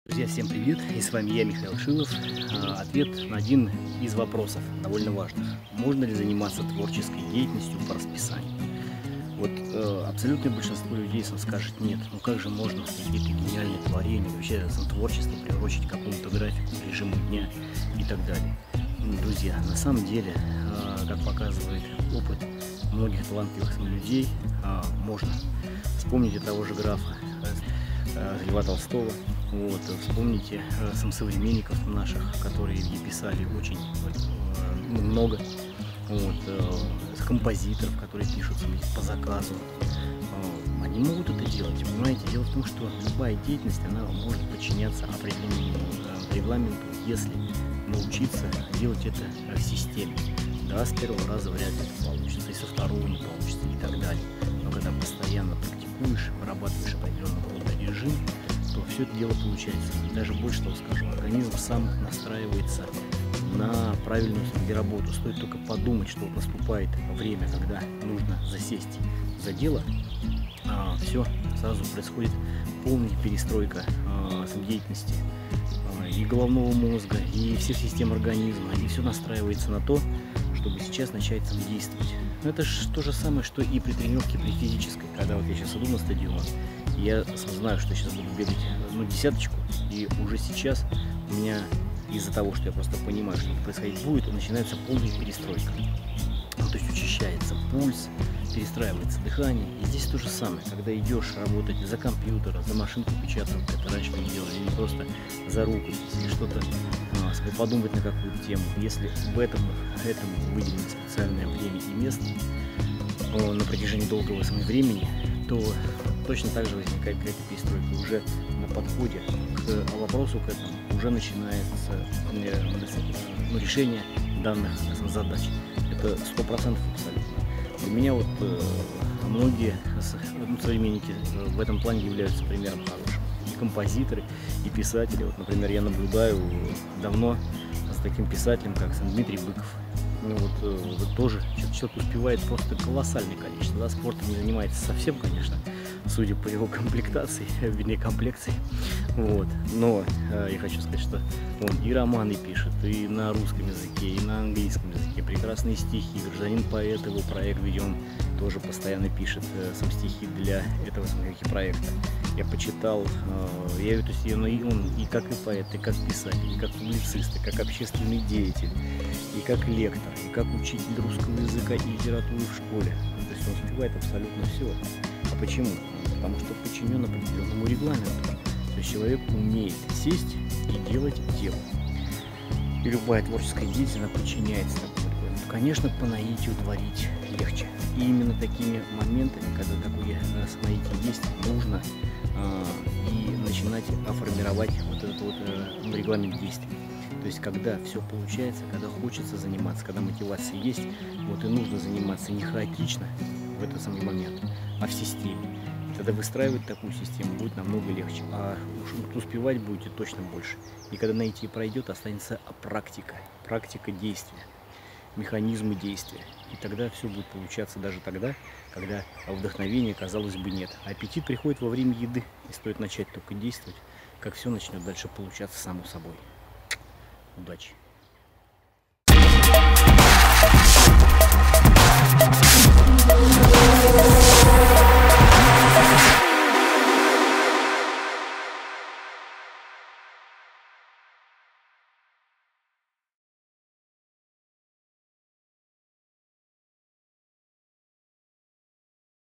Можно ли заниматься творчеством по расписанию
Можно! И даже нужно.
При систематичности это очень помогает успевать много. И даже получается лучше!